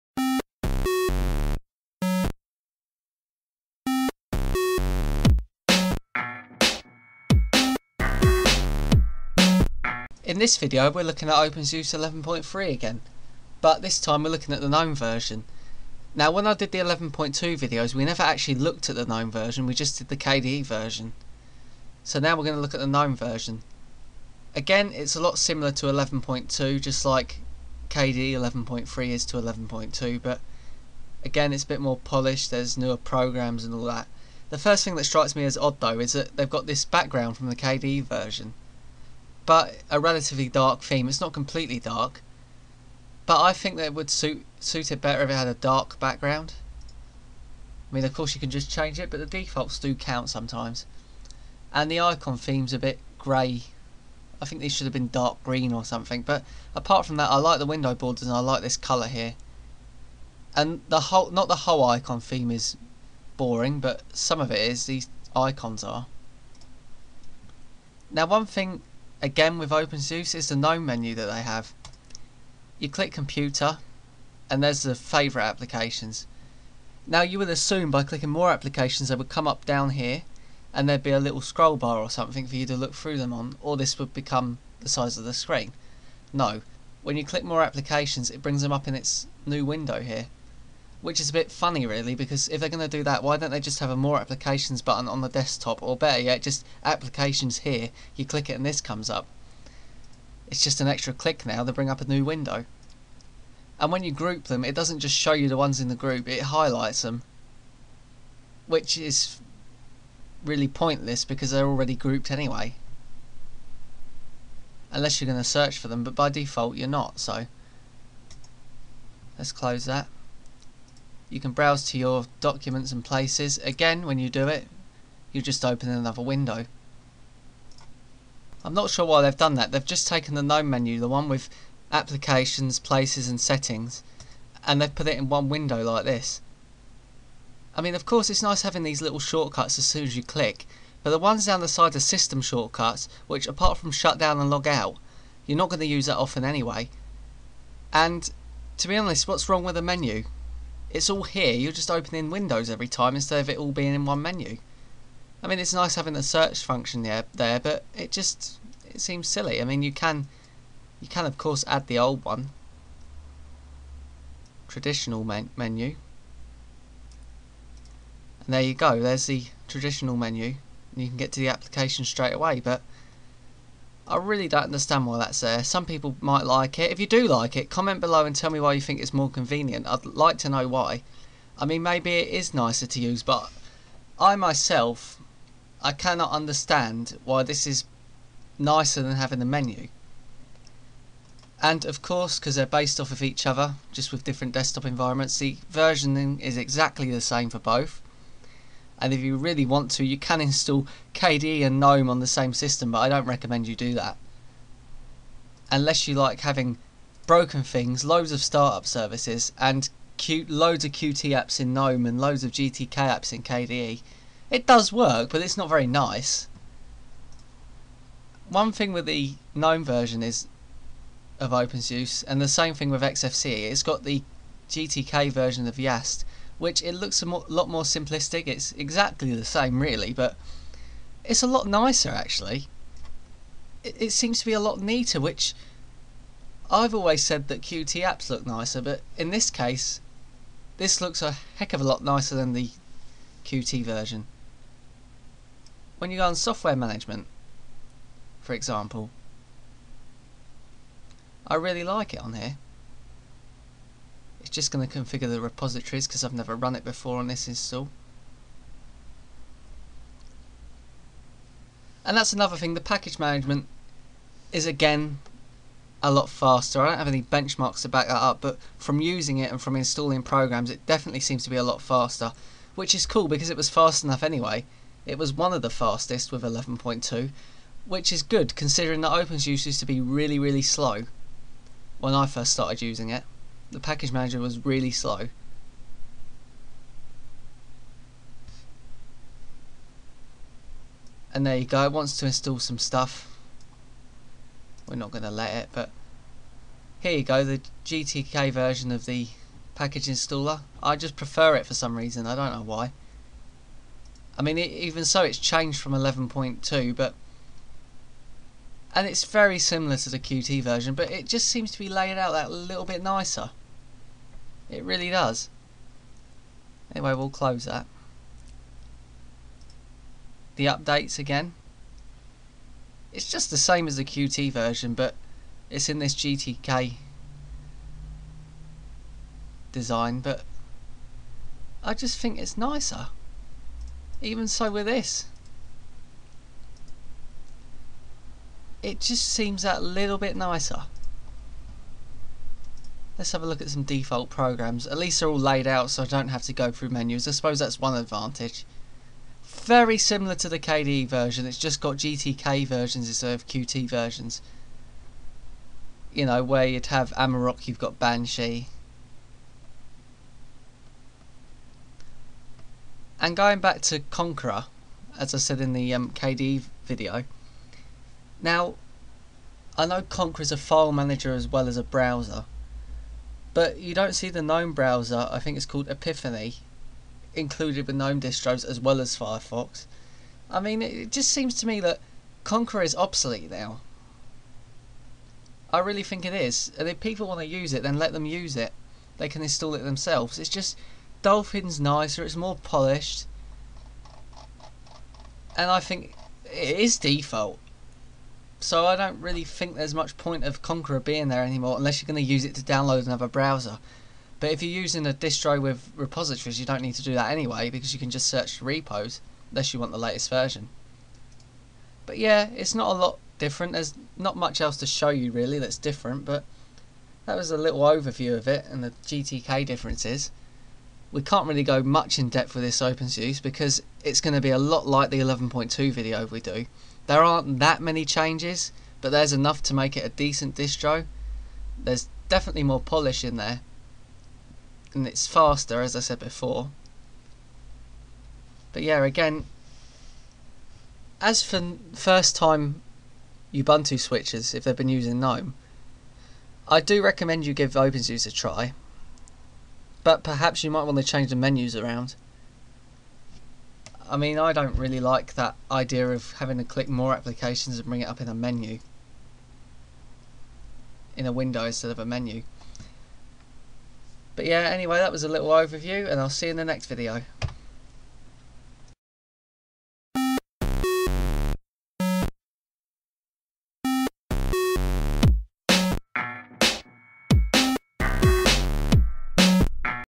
Operating System - OpenSUSE 11.3 Gnome
In this video we look at OpenSUSE 11.3 Gnome. There doesn't seem to be much difference between OpenSUSE 11.2 & 11.3 apart from the under the hood changes and the newer versions of the included software, it's more of a refined release than something new but that's one of the things which makes it so awesome.

This video looks at:

The Live CD
The Live CD Installation
OpenSUSE's Gnome Implementation
GTK Elements
Default Software
The Package Manager